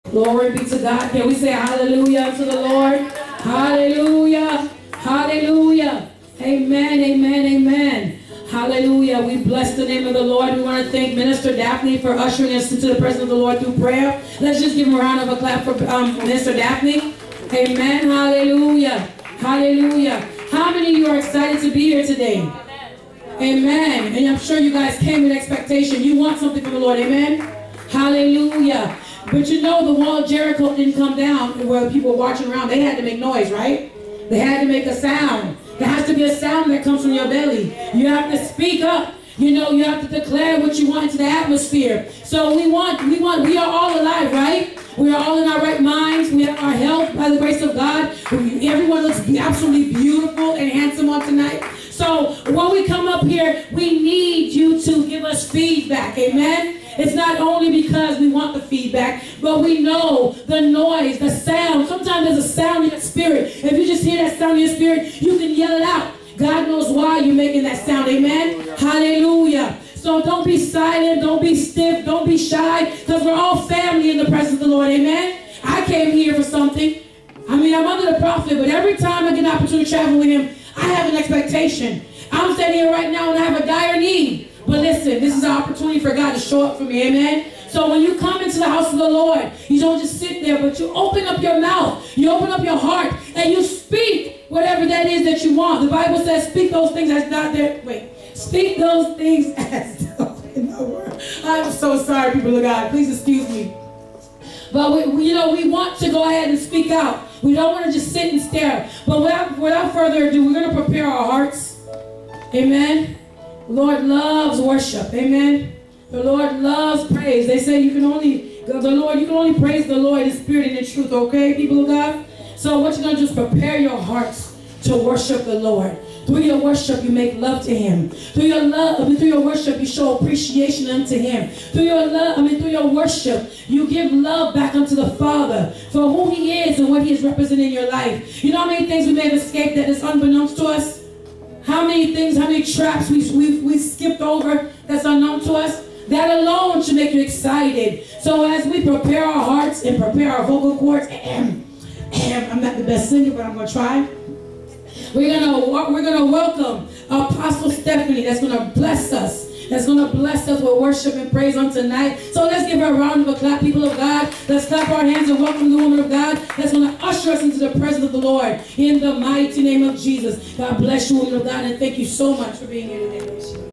06 30 2023, 03408, Evelyn Ceremy ‎Friday, ‎June ‎30, ‎2023, ‏‎8:03:45 PM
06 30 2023, 3 Day Emergency Prrophetic Revival
Host Prophet Sylveser Osei , Gust: pasteur Ena Ulysses
Friday June 30th - July 1st - 2nd , 2023
6212 Silver Star Road Orlando Florida 32808